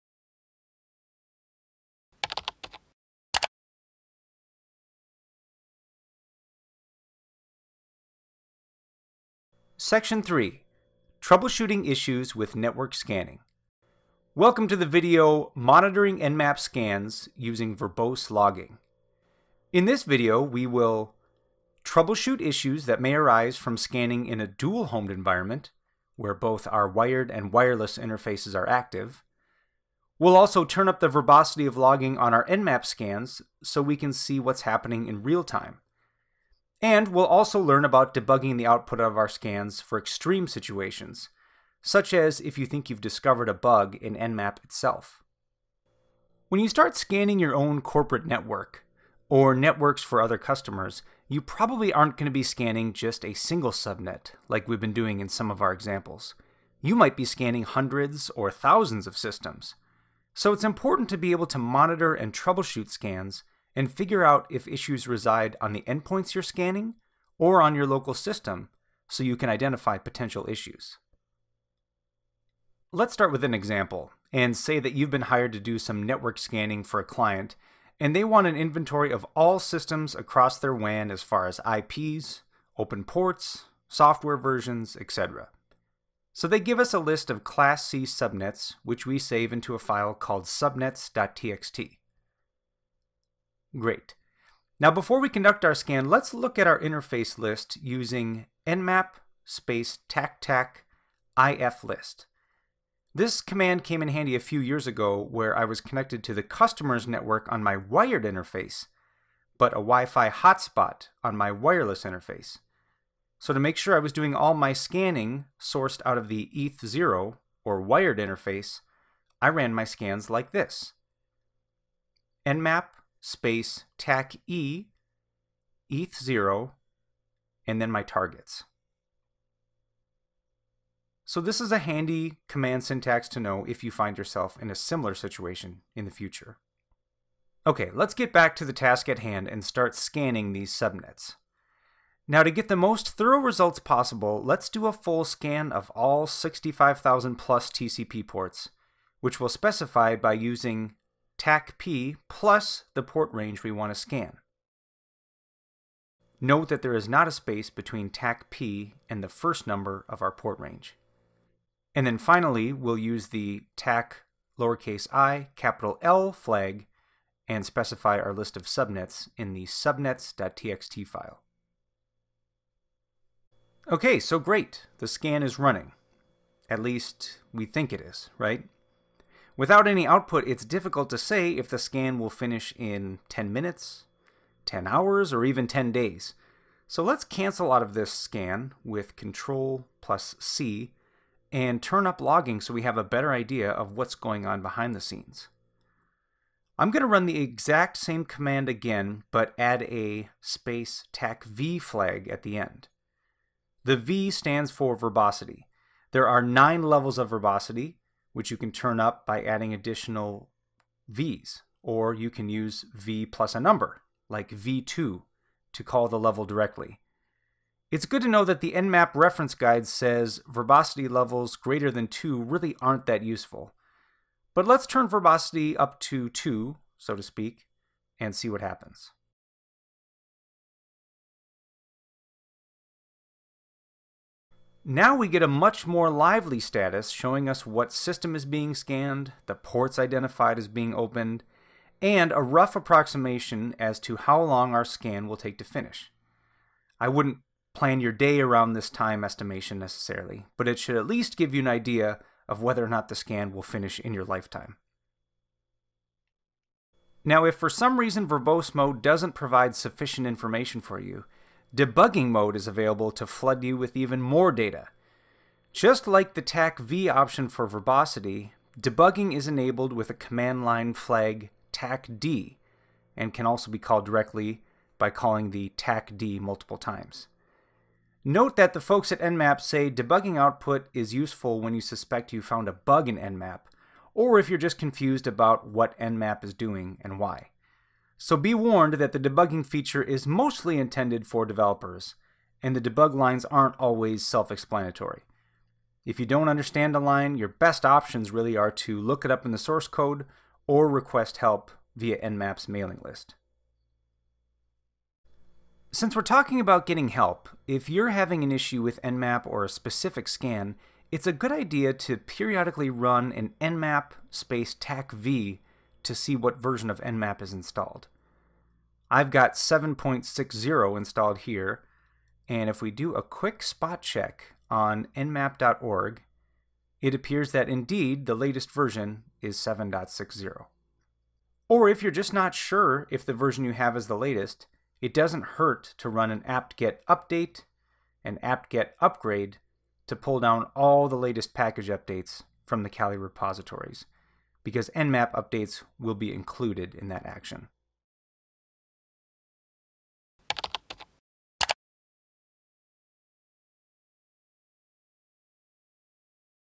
Mastering Kali Linux Network Scanning : Monitoring Nmap Scans Using Verbose Logging | packtpub.com
This video tutorial has been taken from Mastering Kali Linux Network Scanning. You can learn more and buy the full video course